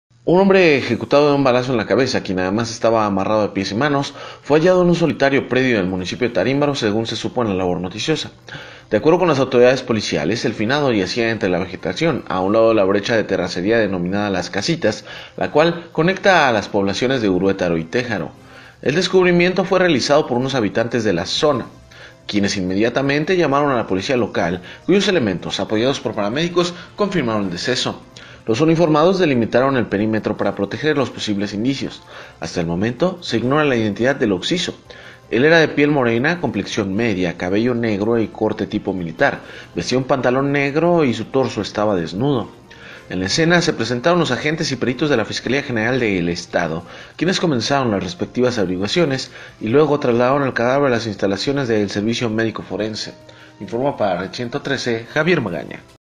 Hallan a un ejecutado en el municipio de Tarímbaro
+ La víctima tenía huellas de tortura.

+ El cadáver estaba a la orilla de un rústico camino localizado entre las poblaciones de Téjaro y Uruétaro.

Tarímbaro, Mich.- 27 de Febrero de 2020.-Un hombre ejecutado de un balazo en la cabeza, quien además estaba amarrado de pies y manos, fue hallado en un solitario predio del municipio de Tarímbaro, según se supo en la labor noticiosa.

De acuerdo con las autoridades policiales, el finado yacía entre la vegetación, a un lado de una brecha de terracería denominada "Las Casitas", la cual conecta a las poblaciones de Uruétaro y Téjaro.

El macabro descubrimiento lo hicieron uno habitantes de la zona durante la mañana de este miércoles e inmediatamente avisaron a la Policía local, cuyos elementos, apoyados por paramédicos, confirmaron el deceso.

Los uniformados delimitaron el perímetro para proteger los posibles indicios. Hasta el momento se ignora la identidad del occiso, él era de piel morena, complexión media, cabello negro y corte tipo militar; vestía un pantalón negro y su torso estaba desnudo.

En la escena se presentaron los agentes y peritos de la Fiscalía General del Estado (FGE), quienes comenzaron las respectivas averiguaciones y luego trasladaron el cadáver a las instalaciones del Servicio Médico Forense (Semefo).